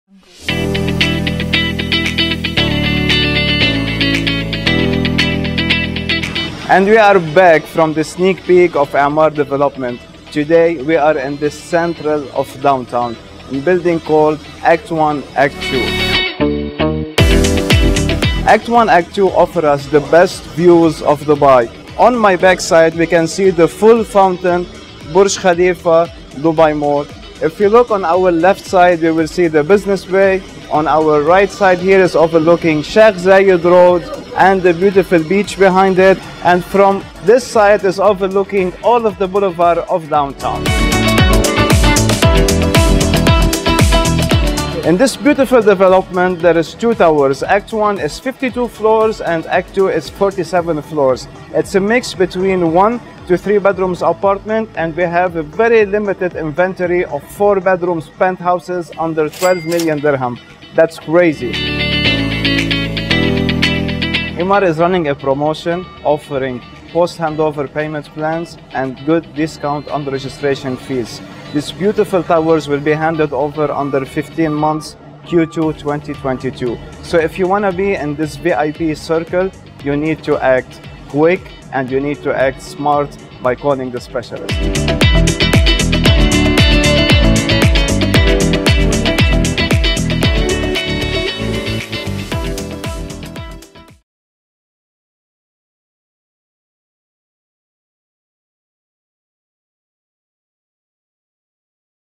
Downtown Dubai Apartments | Emaar Apartments | Act 1 Act 2
ACT I ACT II: Emaar's most stunning apartments in Downtown Dubai. Move into these exquisite homes in 15 months! Act Now.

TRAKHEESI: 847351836